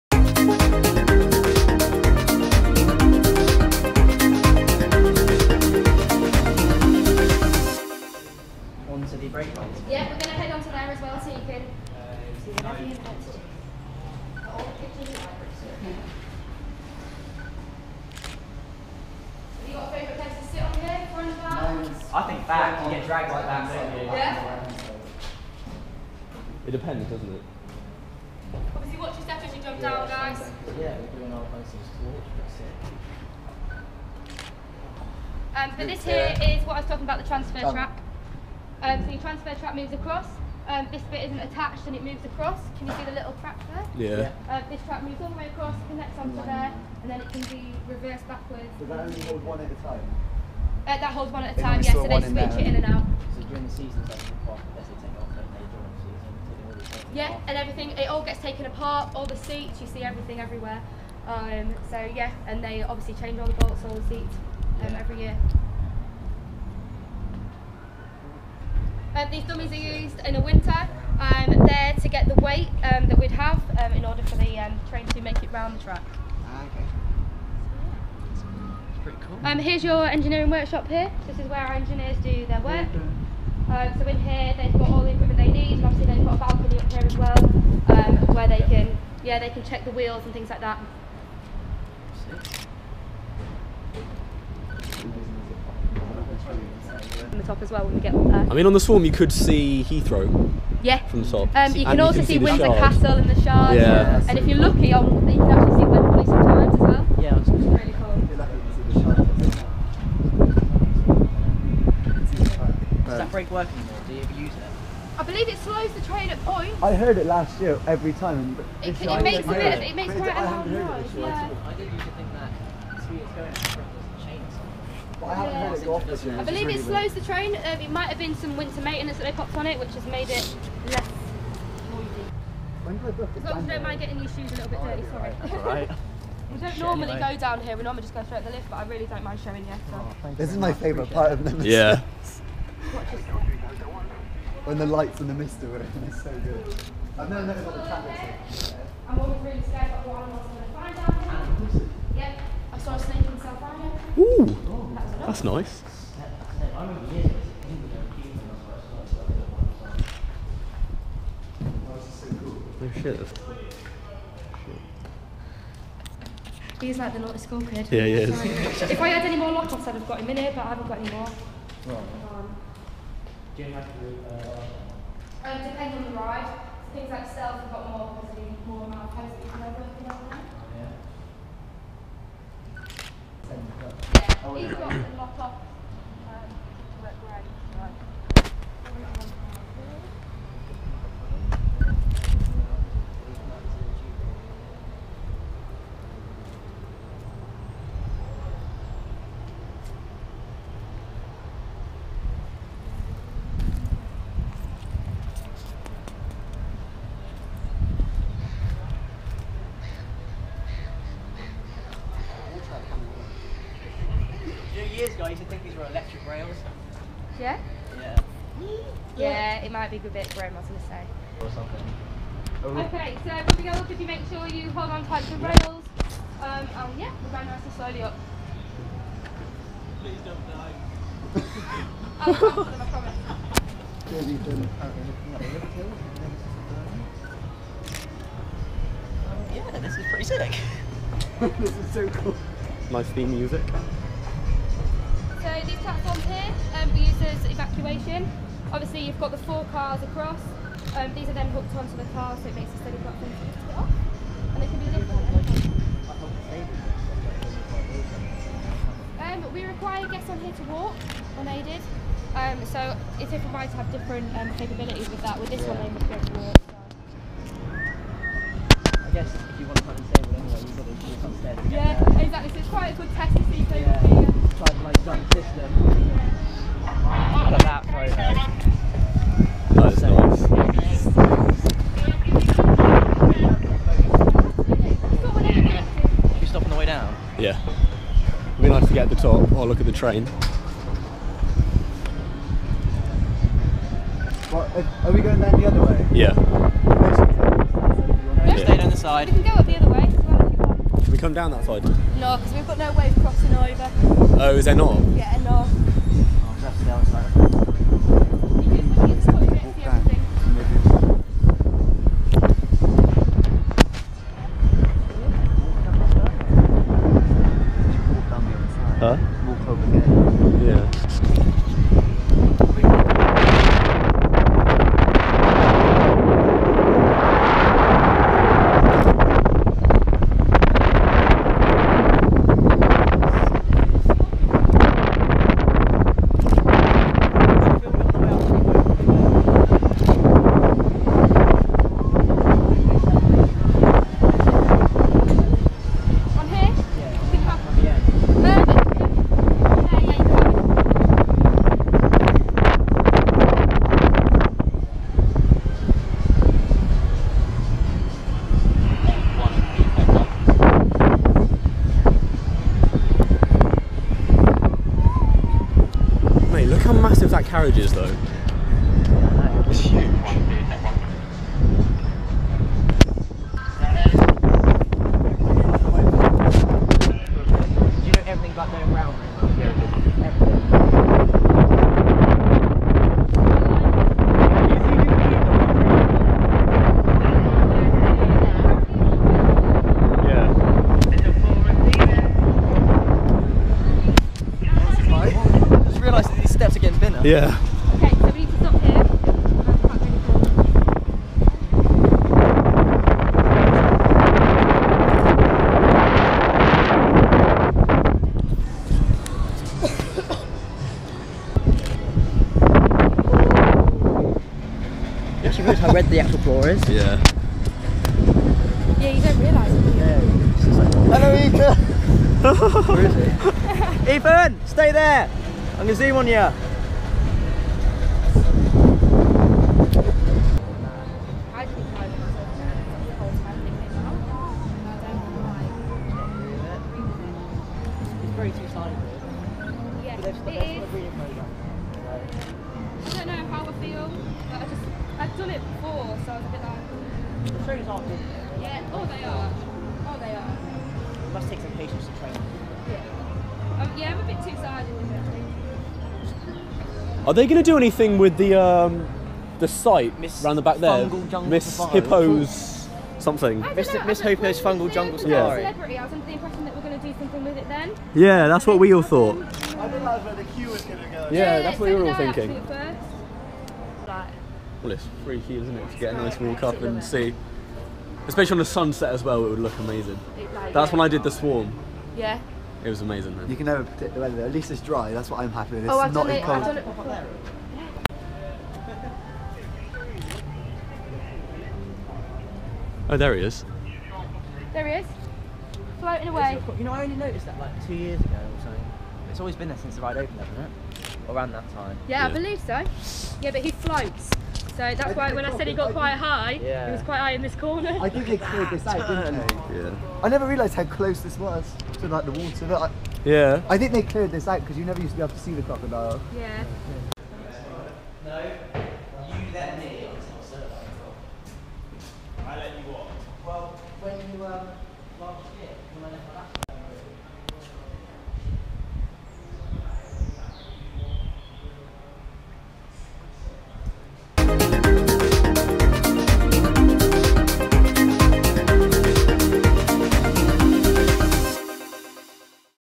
Nemesis Inferno Behind The Scenes and Lift Hill Walk - Thorpe Park Resort
Come with us as we take a behind the scenes look at Nemesis Inferno and Lift hill walk at THORPE PARK Resort.